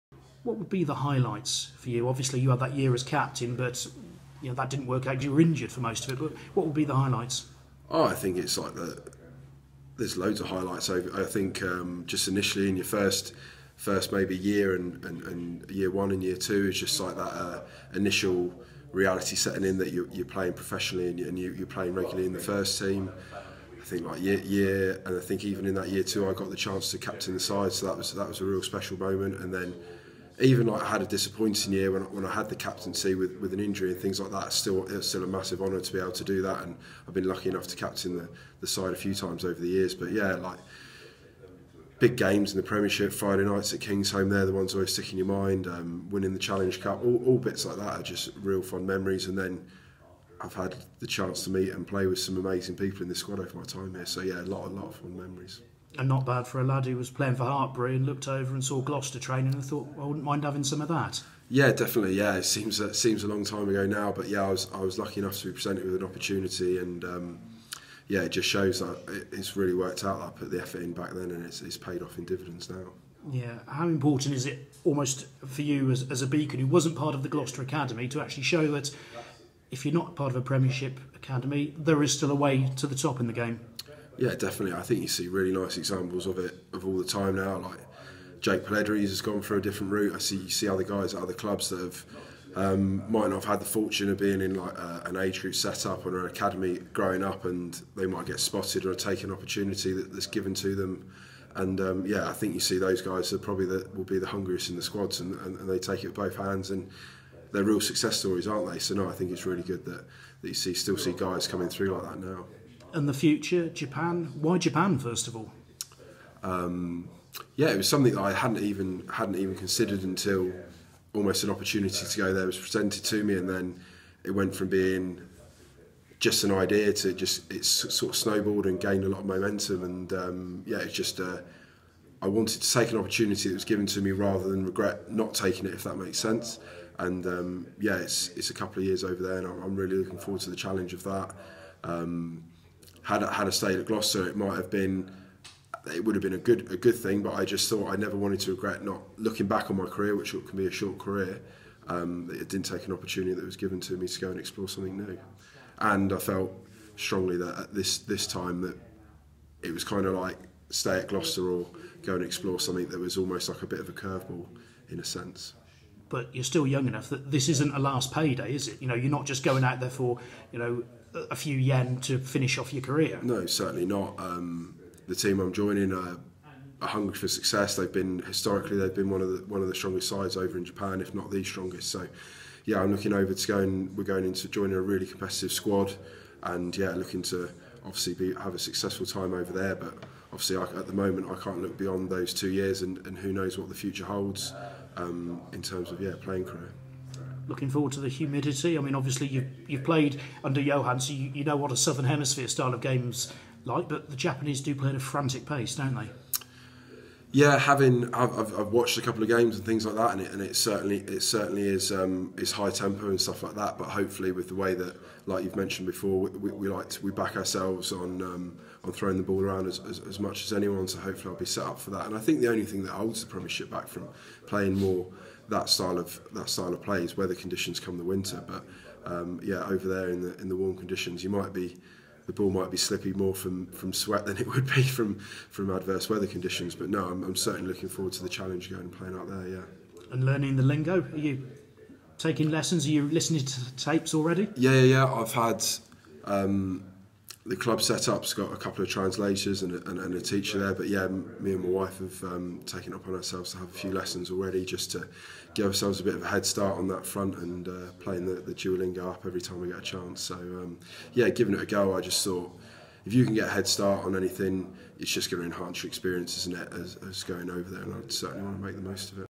Tom Savage on leaving Gloucester
Gloucester lock & former captain Tom Savage looks back on his eight years at the club and joining Suntory Sungoliath in Japan.